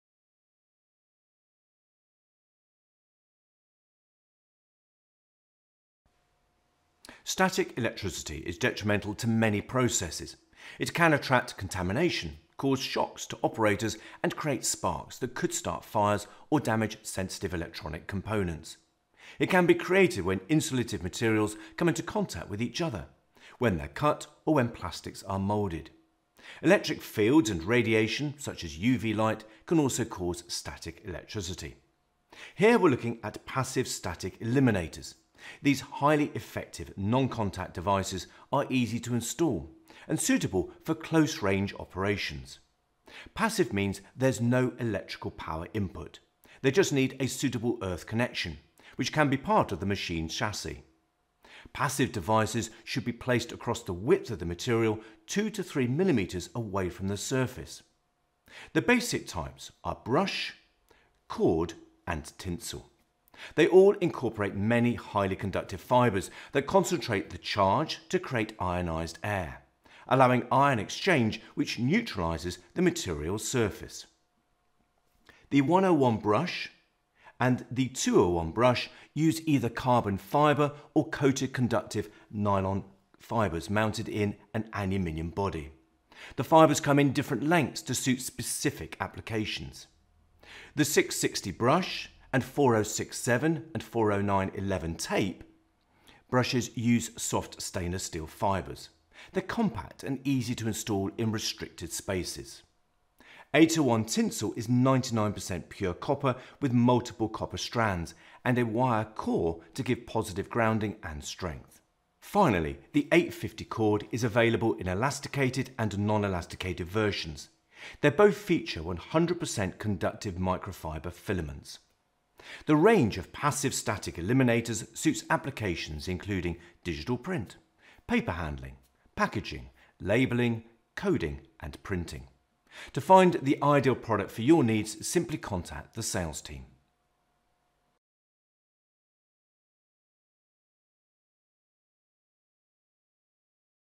How Passive Static Eliminators Solve Static Issues: Applications, Types, and Benefits Explained
Static electricity can disrupt many industrial processes, leading to contamination, shocks, and even fire hazards. It is a common issue caused by insulative materials coming into contact with each other, being cut, or during plastic moulding. Other sources include electric fields and radiation like UV light.

In this video, we explore our passive static eliminators - innovative non-contact devices designed to neutralize static electricity safely and efficiently. These devices are easy to install, require no electrical power input, and only need a proper earth connection—often part of the machine chassis. These include our 101, 201, 660, 406/7, and 409/11 brushes, 801 tinsel, 850 and 850E cord.

Need help selecting the right passive static eliminator for your specific application? Our expert sales team is here to guide you.